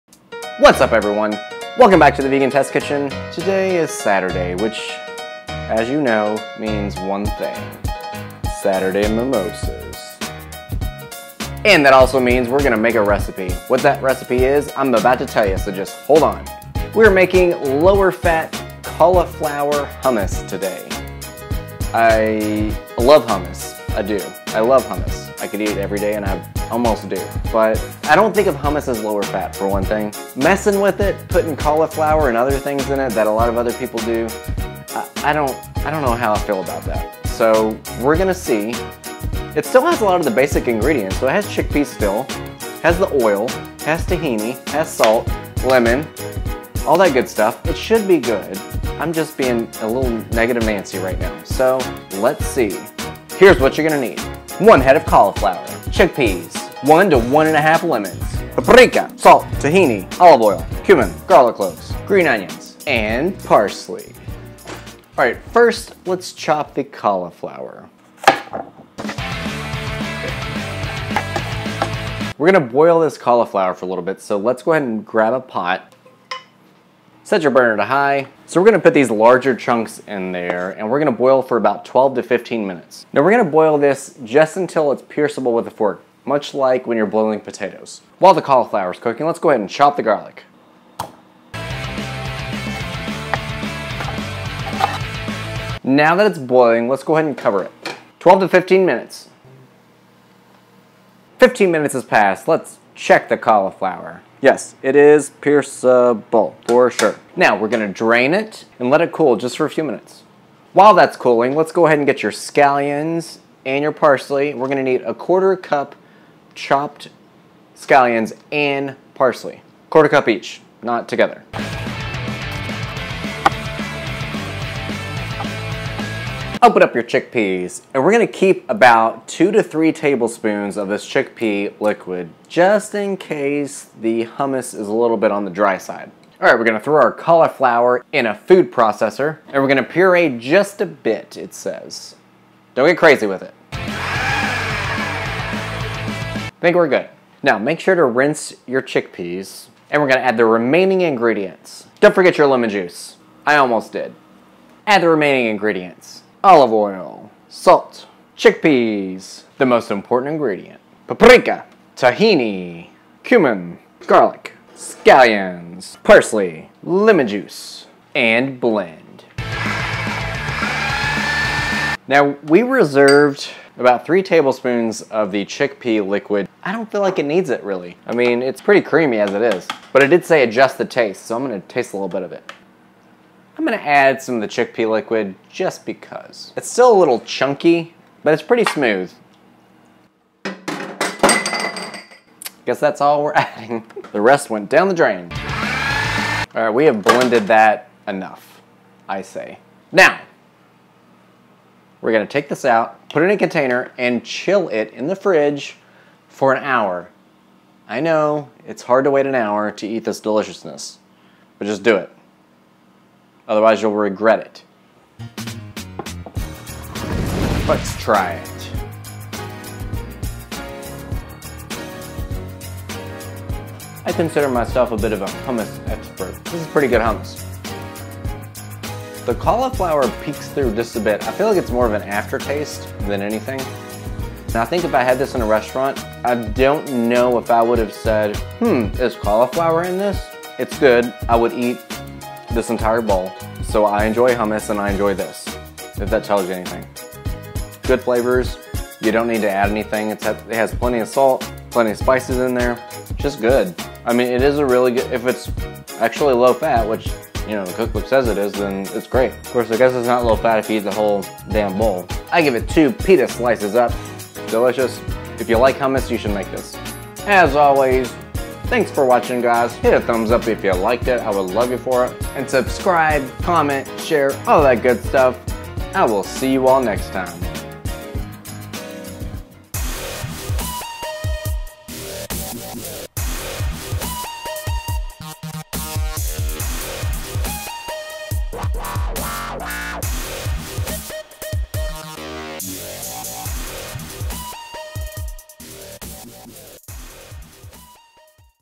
Lower-Fat Cauliflower Hummus Recipe | The Vegan Test Kitchen
Making hummus lower-fat seems almost sacrilegious...but this version from the Veganomicon made with cauliflower still delivers.
*gluten free

Over the course of 2017 I'll be tackling every recipe in one of the most popular, comprehensive vegan cookbooks. Join me (an amateur chef), and cook along in The Vegan Test Kitchen!

w/Eli Haynes, edited by JosephLucas

Ingredients:
1 head cauliflower (around 2 lbs), chopped into large pieces
2 or 3 cloves garlic, chopped
1 can chickpeas, washed/drained (with 3 tbsp liquid reserved)
2 tbsp tahini
1 tbsp olive oil
1/4 cup chopped scallions
1/4 cup loosely packed fresh parsley
1/2 tsp salt
2 tsp cumin
1/4 tsp paprika
2-3 tbsp lemon juice